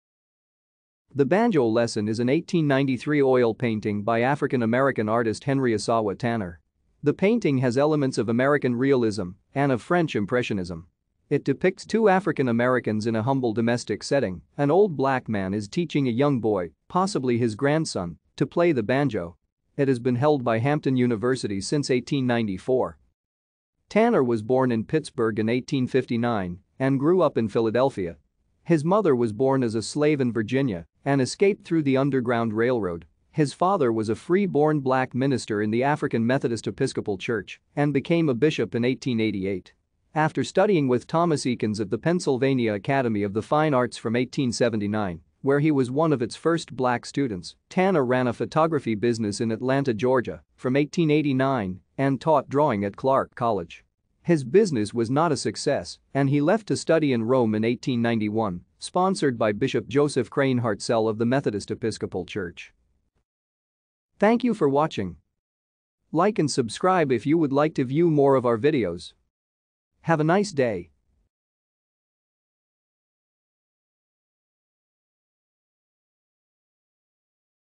The Banjo Lesson
The Banjo Lesson is an 1893 oil painting by African-American artist Henry Ossawa Tanner.  The painting has elements of American Realism and of French Impressionism.  It depicts two African-Americans in a humble domestic setting: an old black man is teaching a young boy – possibly his grandson – to play the banjo.  It has been held by Hampton University since 1894.